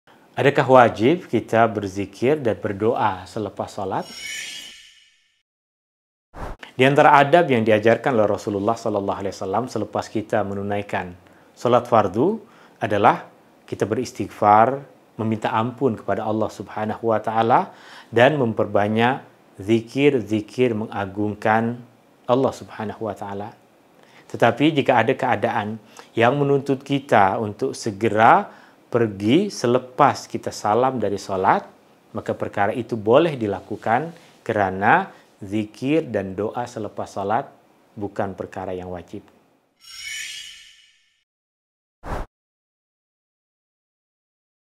Do”s & Don’ts In Solat (Wajibkah Doa & Berzikir Selepas Solat?) | Ustaz Barry
Wajibkah Doa & Berzikir Selepas Solat?

Haa mesti ramai nak tahu kan? Jom sama-sama kita dengarkan penjelasan daripada Ustaz Barry✨